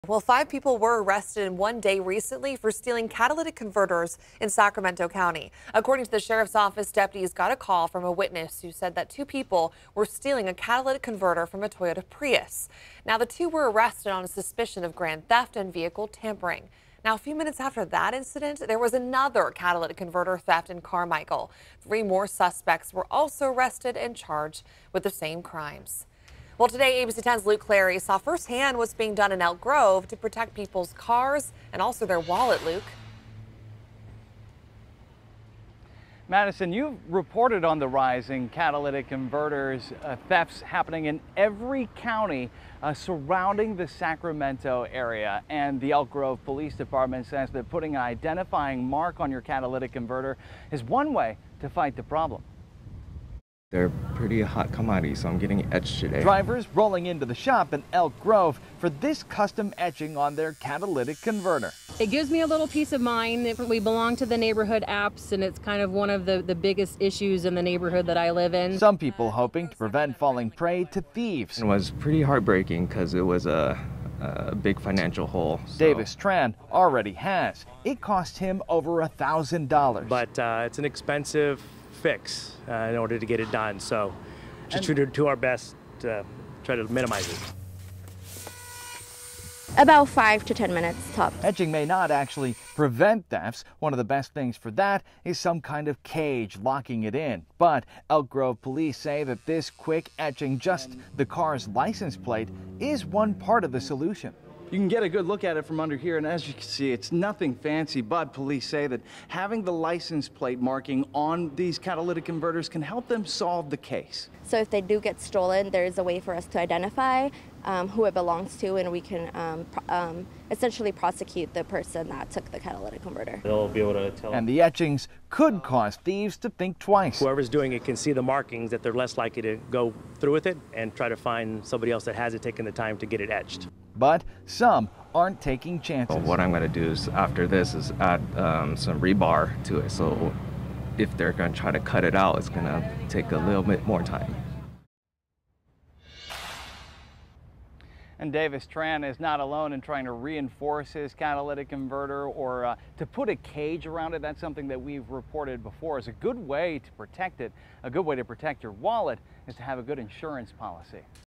How to stop someone from stealing your catalytic converter
Five people were arrested in one day for stealing catalytic converters in Sacramento County. According to a Facebook post from the Sacramento County Sheriff's Office, deputies were called to a business park along the 5000 block of Madison Avenue in Sacramento after receiving a call from someone who was watching "two subjects steal a catalytic converter from a Toyota Prius." Upon arrival, deputies located one suspect acting as a lookout while another was seen under the Prius removing parts.

Luke Cleary was in Elk Grove getting a first-hand look at what's being done to stop catalytic converter theft.